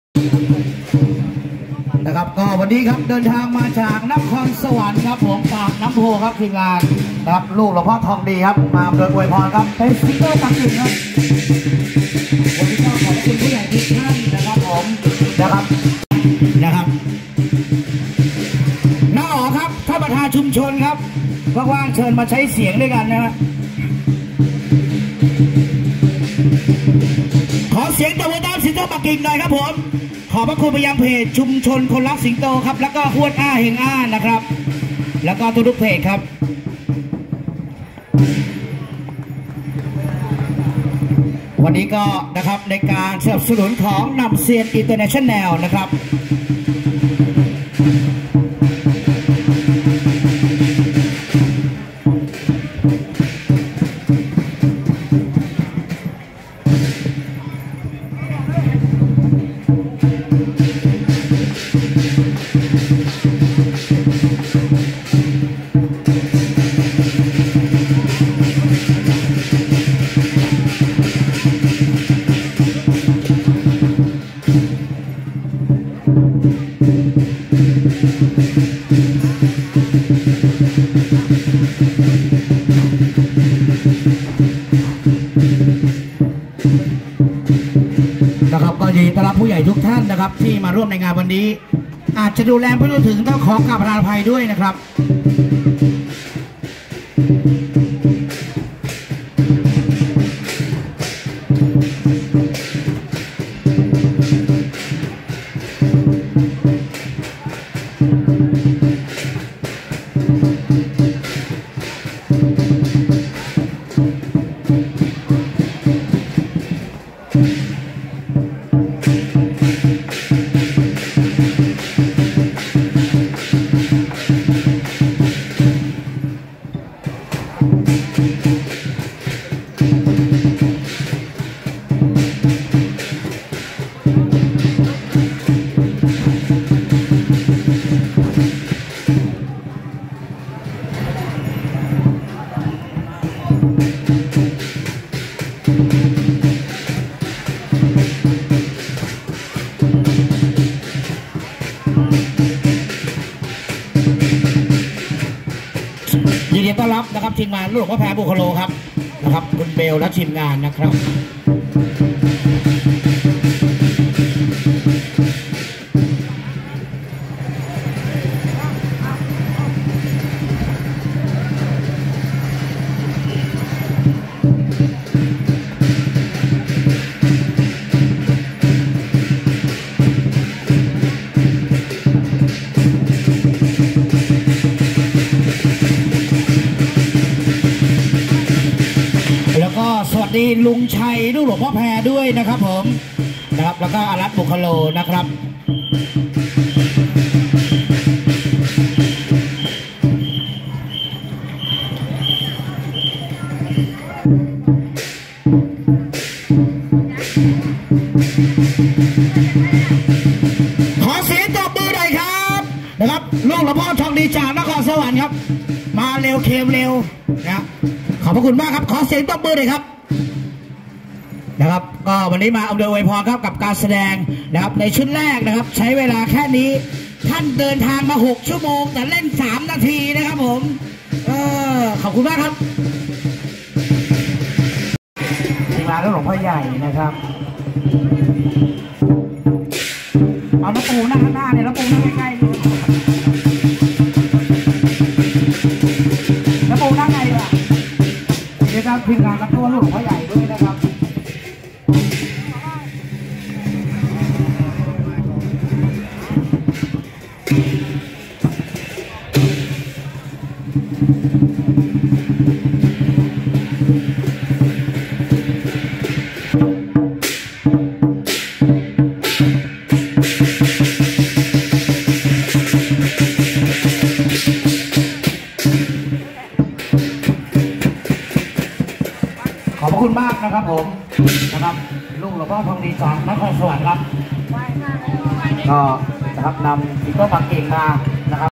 สิงโตปักกิ่งลูกหลวงพ่อทองดี นครสวรรค์งานไหว้ครูครบรอบ55ปีคณะสิงโตลูกหลวงพ่อโอภาสีNamsiengThailand 南仙
งานไหว้ครู คณะสิงโตลูกหลวงพ่อโอภาสี
จันทร์ที่ 9 มกราคม 2566
สถานที่จัดงาน : ศาลเจ้าโกบ๊อ

- คณะสิงโตลูกท้าวเวสสุวรรณโณ
- คณะสิงโตปักกิ่งลูกหลวงพ่อทองดี นครสวรรค์
- คณะสิงโตศิษย์เครือวัลย์
- คณะสิงโตศิษย์เจ้าแม่กวงอิม
- คณะสิงโตเชียงแสน
- คณะสิงโตจี่เซียนไทยแลนด์
- คณะสิงโตอี้เฉียน
- คณะสิงโตศิษย์พ่อปู่เขาคลุกคลี กาญจนาบุรี
- คณะสิงโตศิษย์ปู่ย่าอุดรธานี
- คณะกลองยาวขุนพลเจริญผล